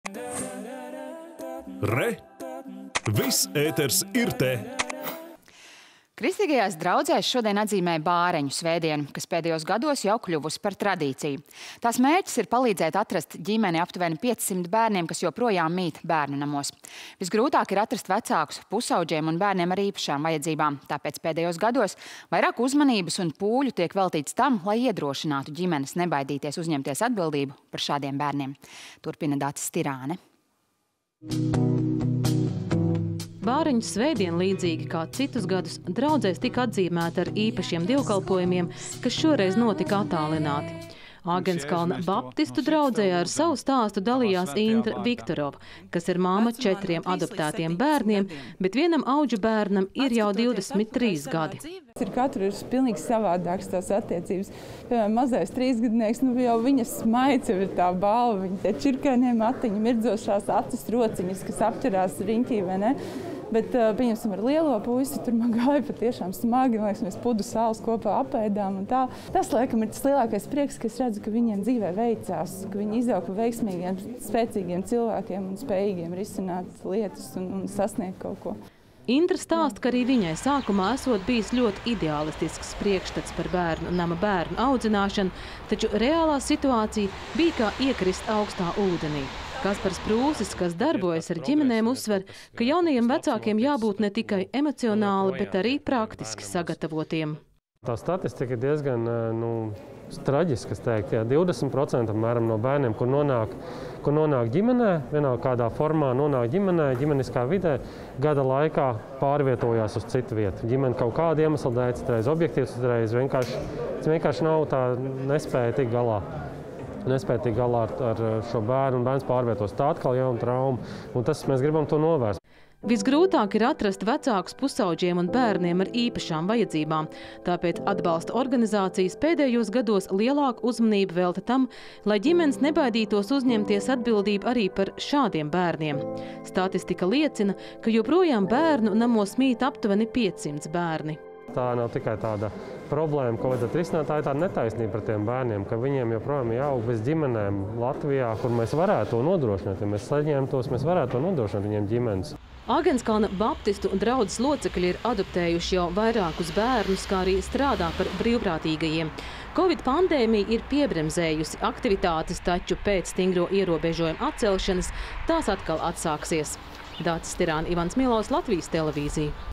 Draudzēs atzīmē “Bāreņu svētdienu”
Kristīgajās draudzēs šodien atzīmēja Bāreņu svētdienu, kas pēdējos gados kļuvusi jau par tradīciju. Tās mērķis ir palīdzēt aptuveni 500 bērniem, kas joprojām mīt bērnu namos, atrast ģimeni. Visgrūtāk ir atrast vecākus tieši  pusaudžiem un bērniem ar īpašām vajadzībām, tāpēc pēdējos gados vairāk uzmanības un pūles tiek veltīts tieši tam, lai ģimenes nebaidīties uzņemties atbildību arī par šādiem bērniem. Turpina Dace Stirāne.